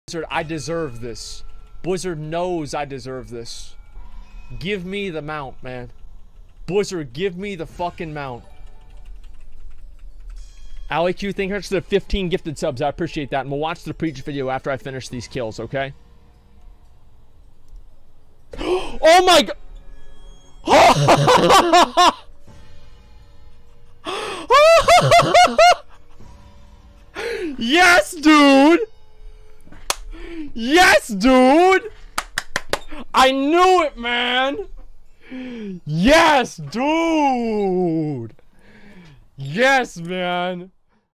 ASMONGOLD GETS CLUTCH OF HA-LI
Asmongold gets the clutch of ha-li mount after leveling up his new character.